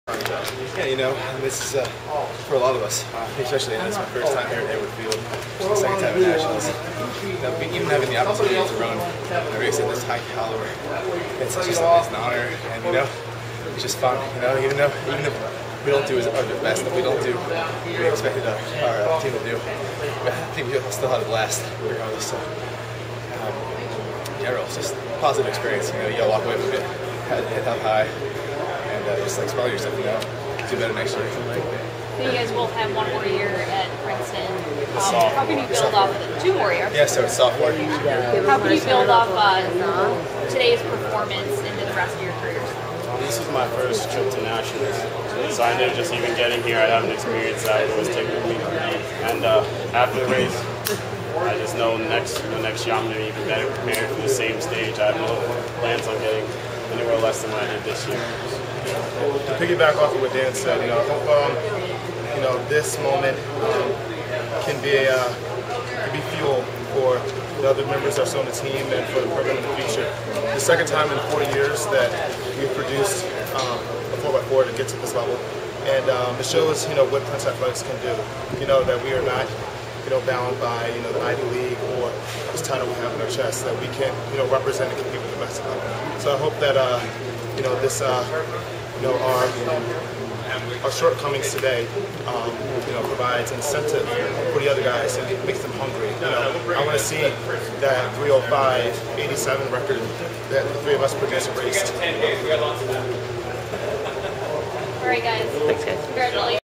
Armory Talk — Princeton 4x400m
For the second time in three years, the Princeton Tigers advanced to the NCAA Championships in the 4x400-meter run, but the race brought a close to two fine careers (Russell Dinkins, Austin Hollimon).